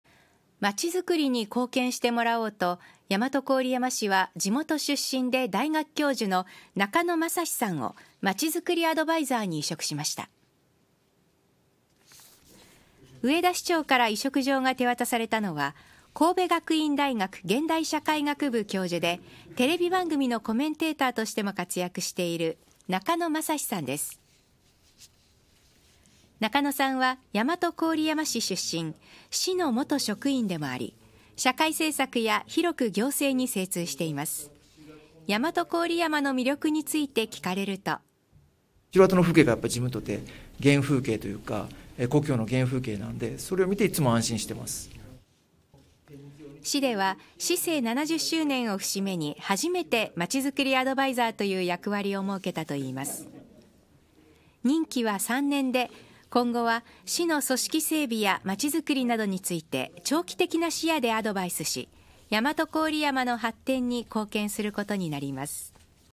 地元出身の中野雅至さん 大和郡山市まちづくりアドバイザーに委嘱
2024/04/09
　まちづくりに貢献してもらおうと大和郡山市は、地元出身で大学教授の中野雅至さんを「まちづくりアドバイザー」に委嘱しました。